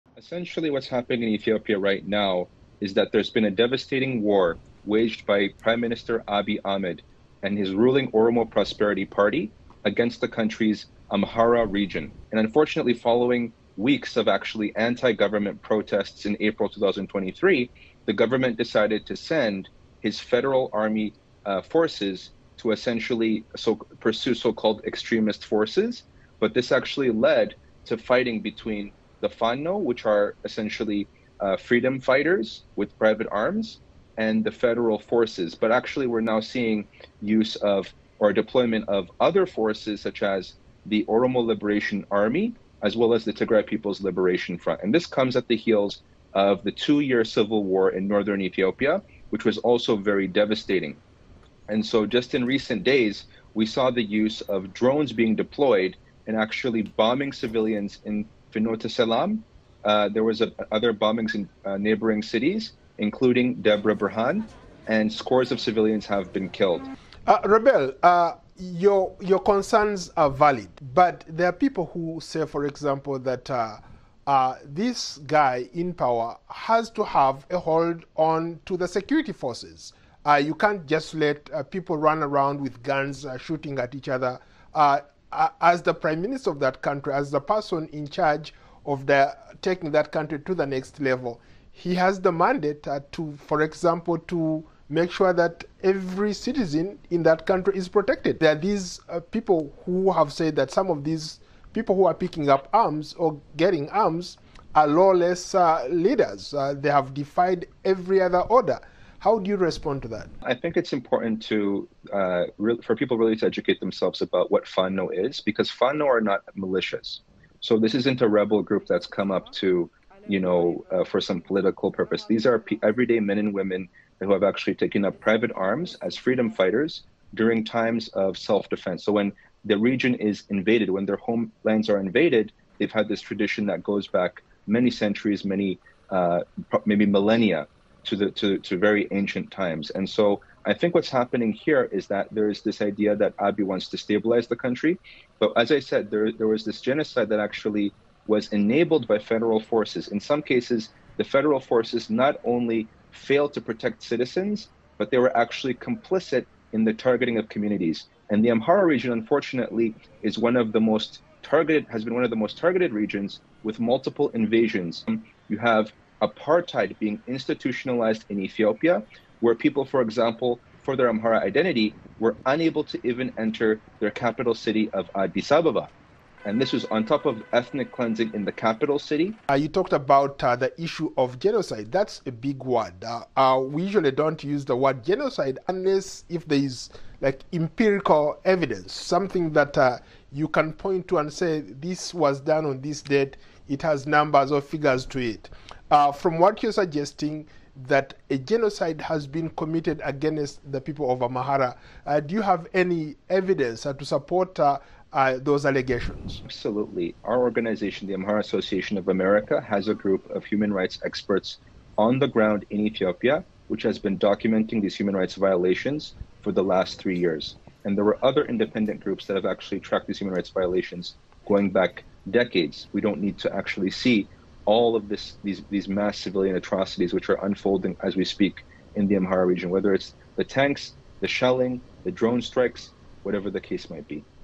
Clashes Continue in Ethiopia’s Amhara Region
In response to the recent clashes between government forces and the Fano militia in the Amhara region, the Ethiopian government has announced a state of emergency in the area. Local reports have indicated that an explosion in Finote Selam, northwestern Ethiopia, resulted in the deaths of at least 26 people and injuries to several others. VOA's Paul Ndiho spoke to Robel Alemu, director of communications for the Amhara Association of America, to gain insight into the unfolding events.